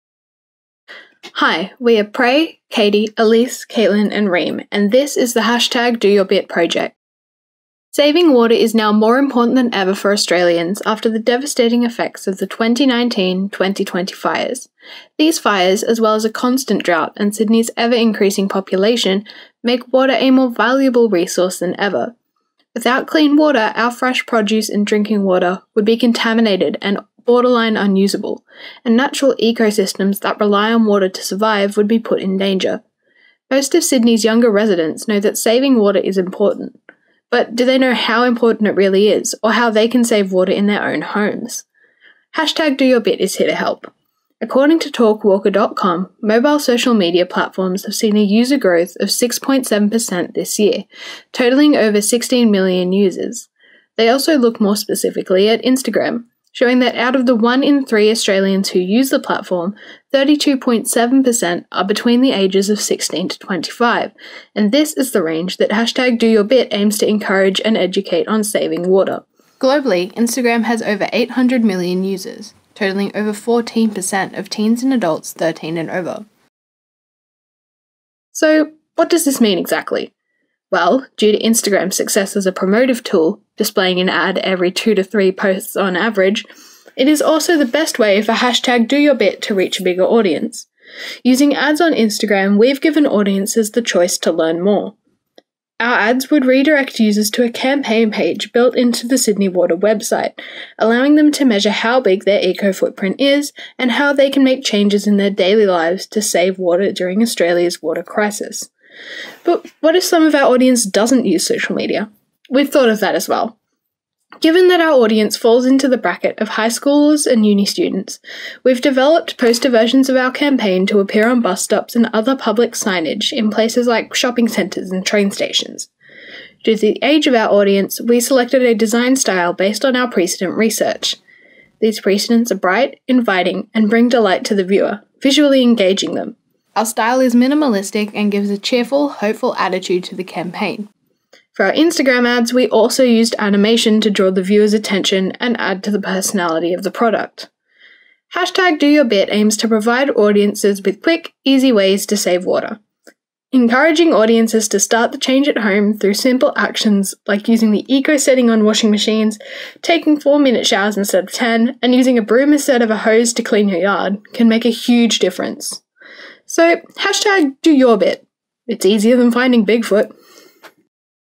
DoYourBit - By Catherine, Reem, Elise, Prabuddhi and Caitlin
Researching the Visual Assessment 3 Group Pitch for Sydney Water.